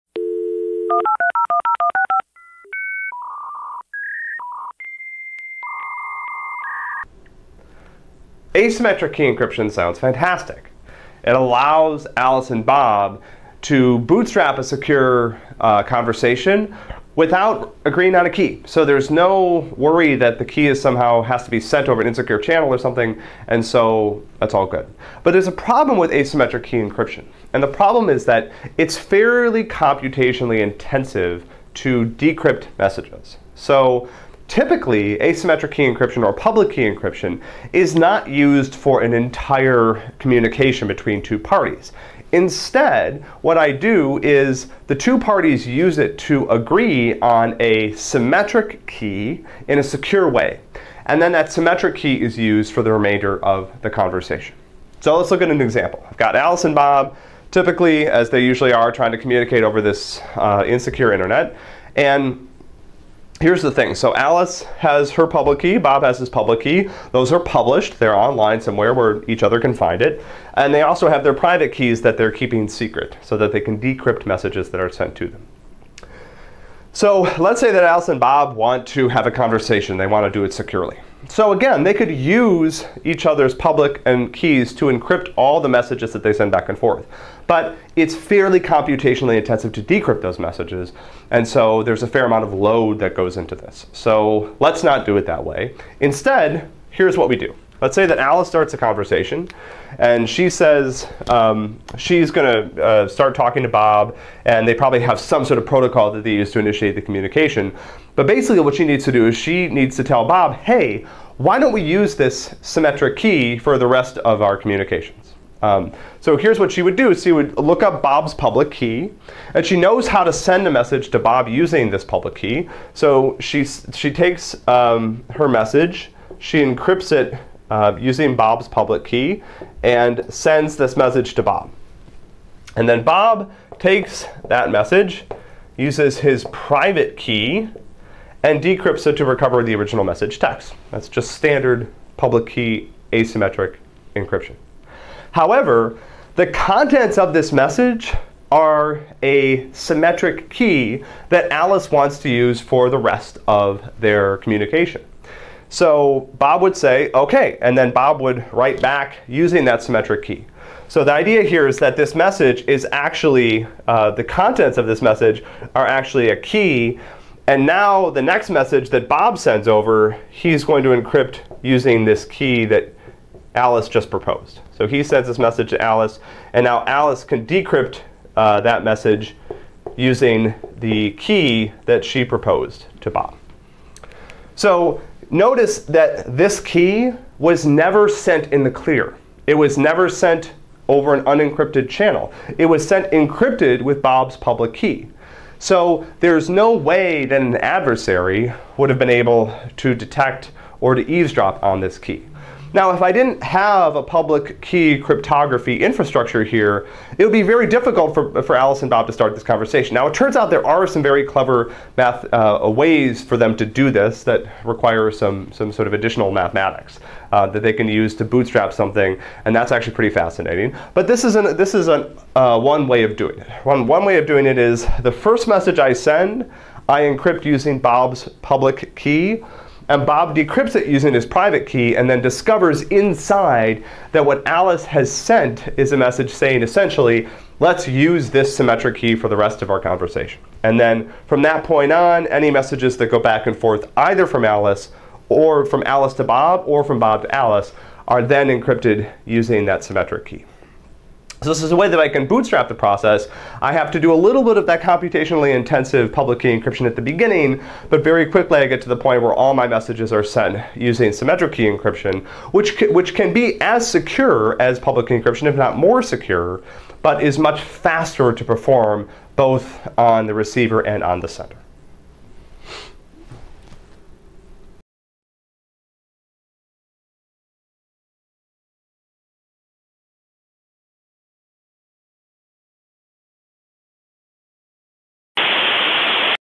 How do two parties exchange keys to communicate securely?
While asymmetric (public-key) encryption does allow two parties to communicate security without exchanging keys, asymmetric encryption requires expensive computation for each message. Symmetric key algorithms are much more efficient. As a result, the two techniques are usually applied to establish and maintain a secure connection. Asymmetric encryption is used to protect the initial part of the connection where symmetric keys are exchanged. Once communicating parties agree on symmetric keys, symmetric encryption is used for the remainder of the communication.